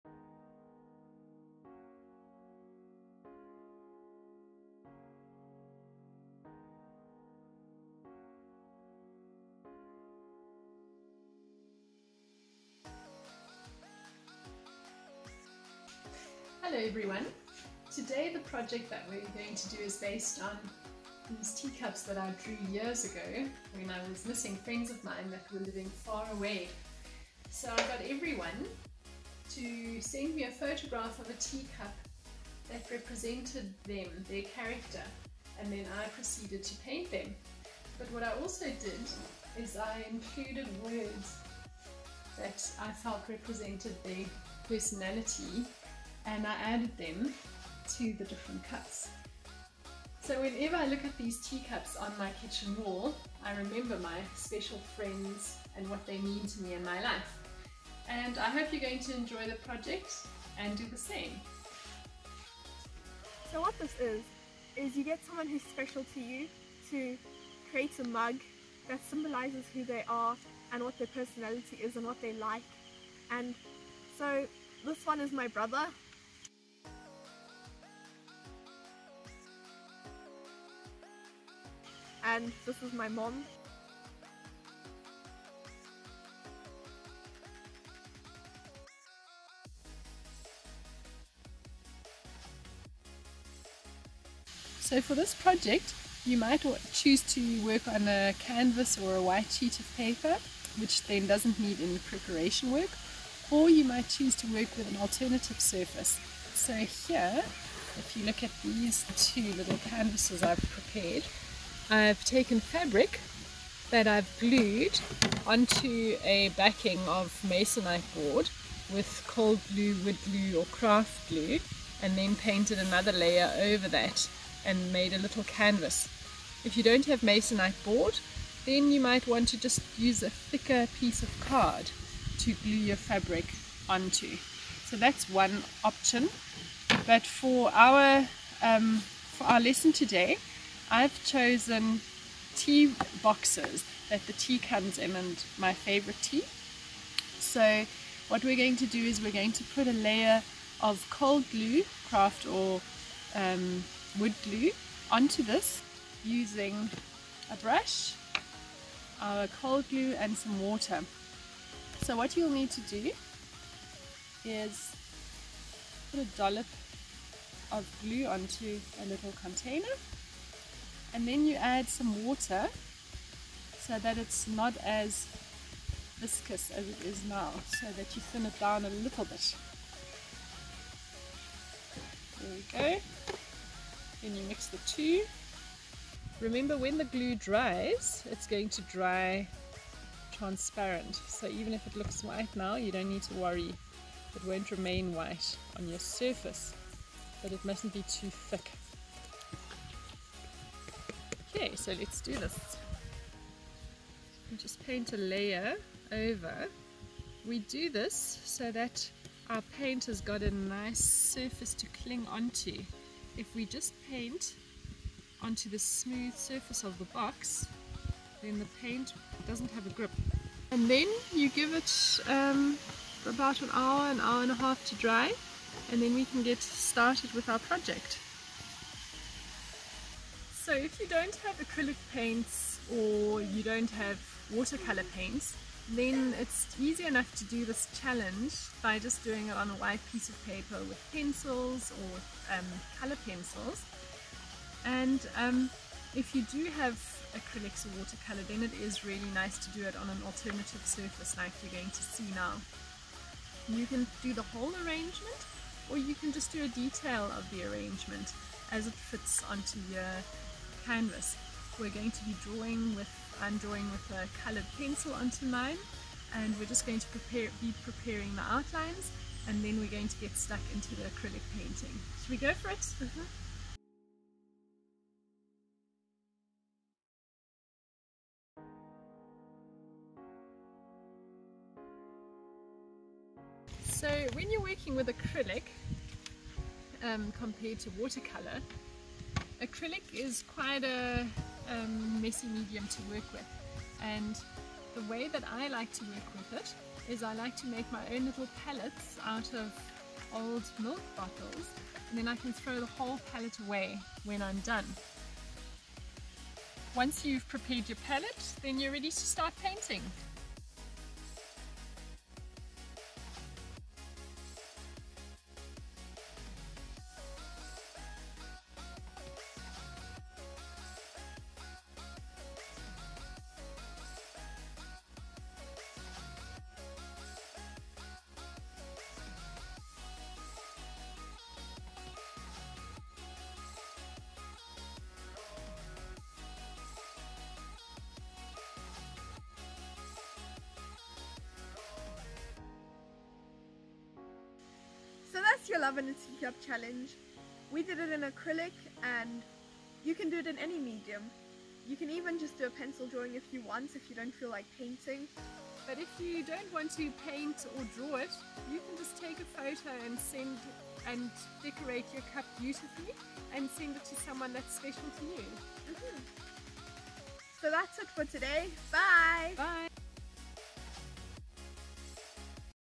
Love in a Teacup (or mug) Challenge | Unleash your creative side
A beautiful challenge to try with your friends! Get your friend to decorate a cup/mug in a way that allows their personality to shine through, and you turn it into an acrylic masterpiece. This artwork can be done on alternative surfaces, such as a tea box or a fabric canvas. Sure to be lots of fun, and a lovely reminder of your friend, should you happen to part ways.